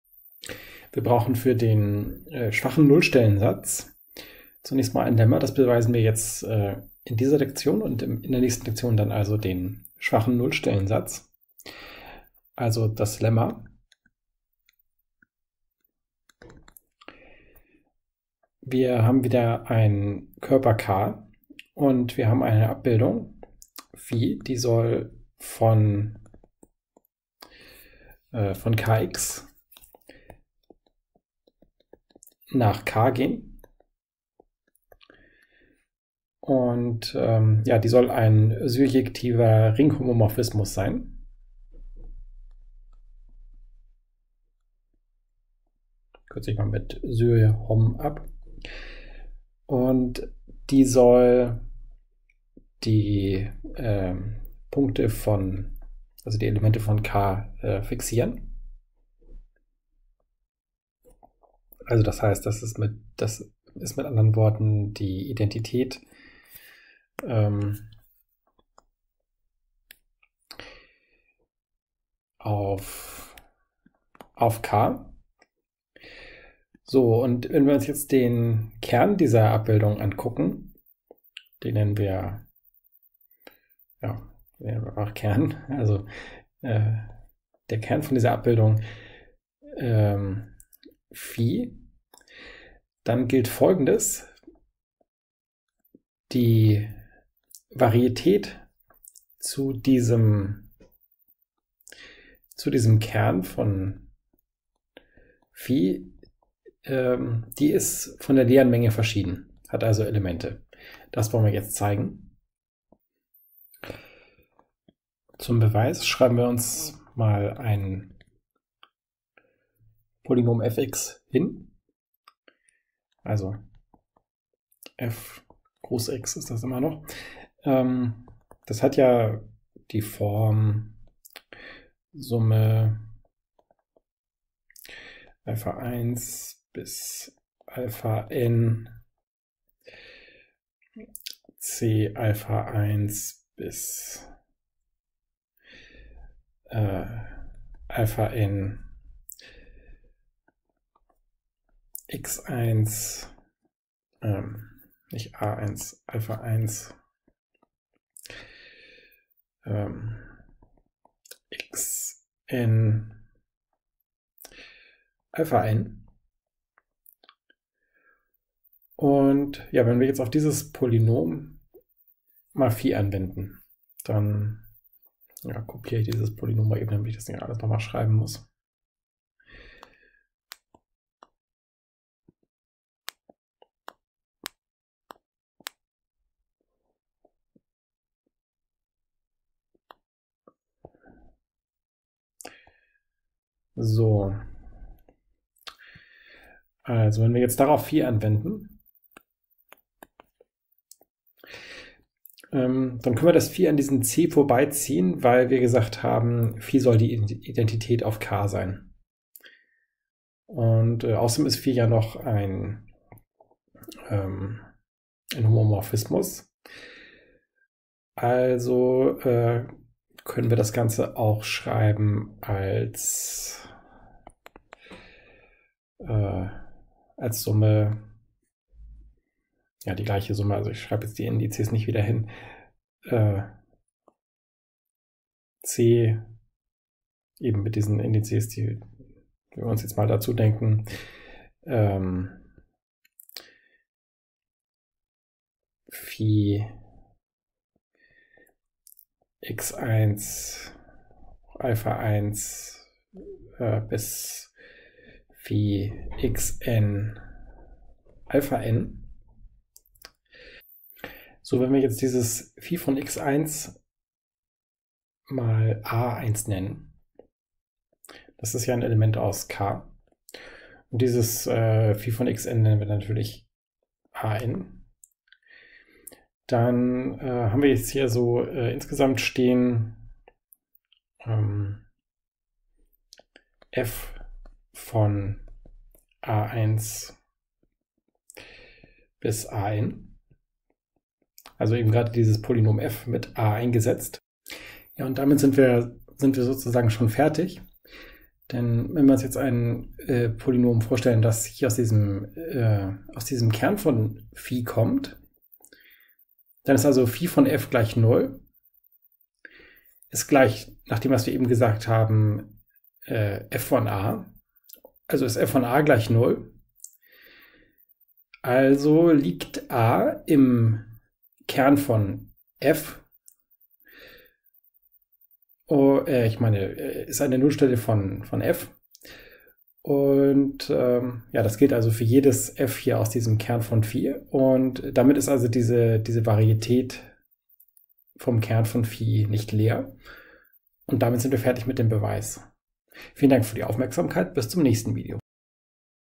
Algebraische Geometrie: Kern hat nichttrivale Varietät Mathekanal
Vorbereitung des schwachen Nullststellensatzes: Wir betrachten einen Homomorphismus von k[X] nach k der auf k und die zum Kern gehörige Varietät. Was ist eigentlich die i-te Wurzel aus i ? Jetzt ansehen